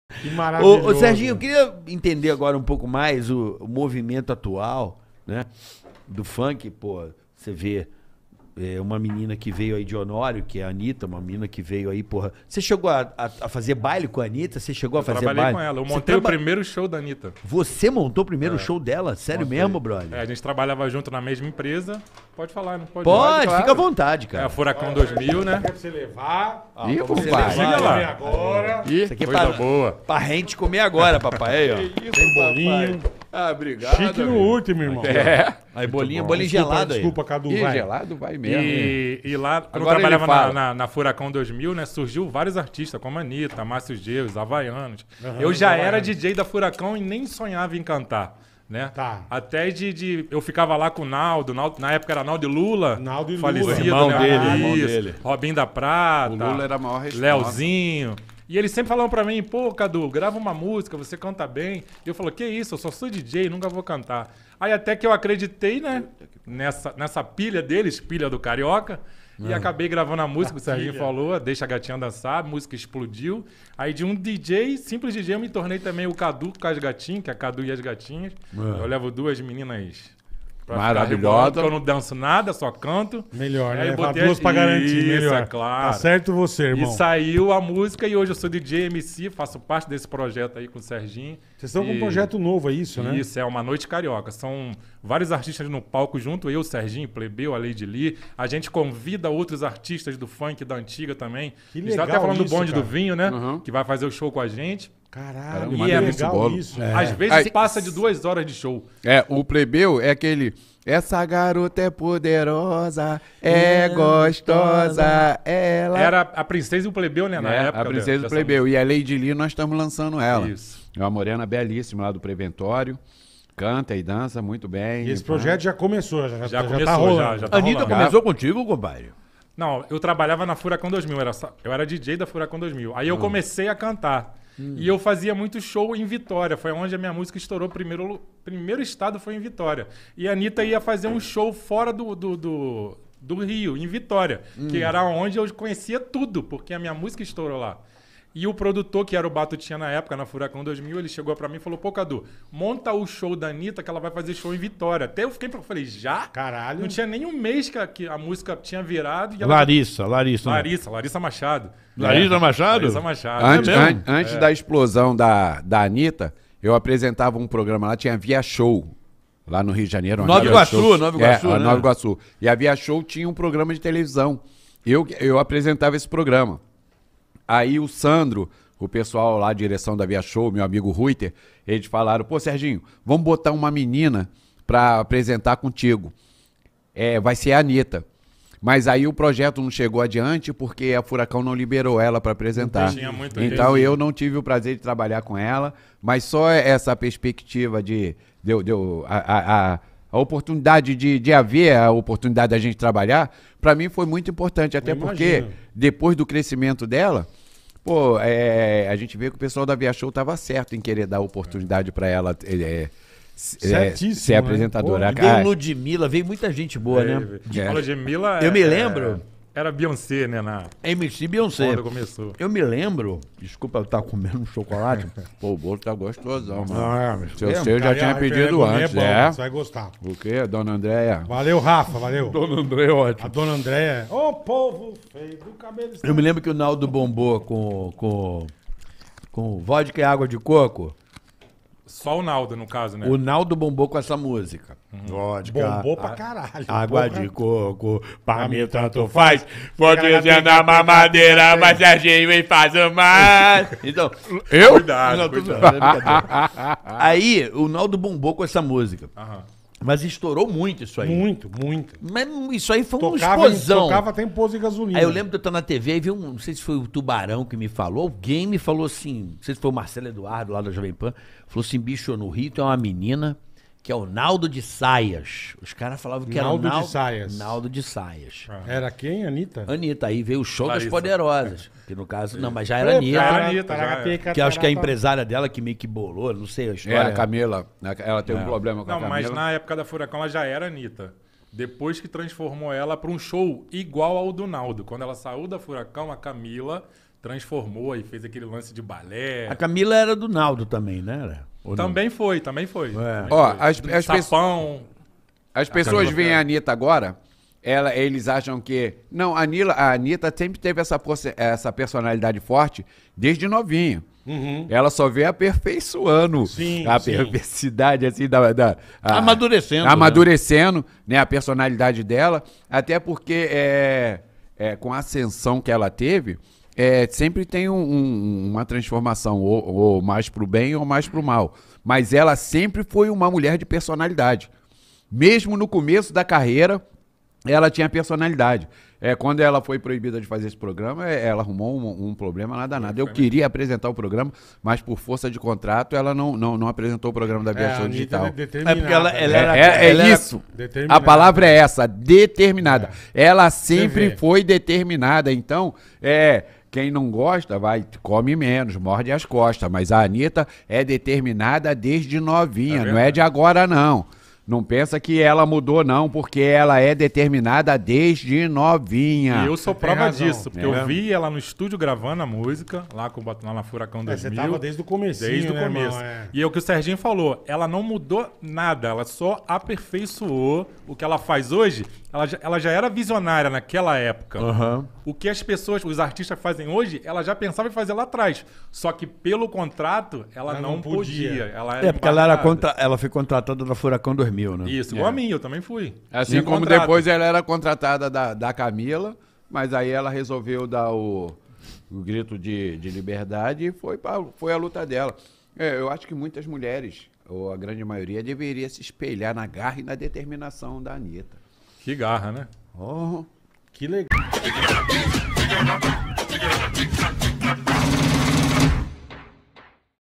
MONTEI O 1º SHOW DA ANITTA | MC SERGINHO - TICARACATICAST
O MC Serginho, funkeiro famoso dos anos 2000 foi um dos entrevistados pelos nossos queridos Bola e Carioca no Ticaracaticast.